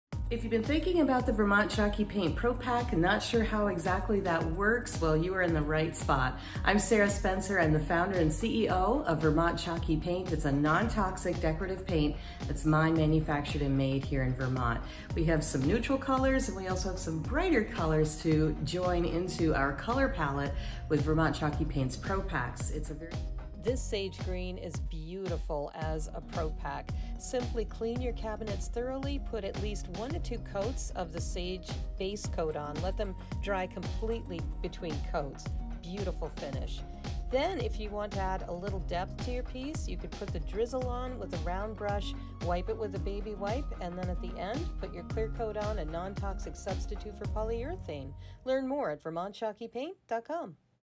Blending & Distressing Sage Green w/ Kitchen Cabinet Makeover Kit
This simple kit helps you distress and antique your cabinets in 4 EASY steps! Buy your ProPack and transform your cabinets this weekend!

- 1 Quart Sage Green
- 1 Quart Clear Coat
- 1 Half-Pint Drizzle
- 1 Round Brush
- 1 Foam Brush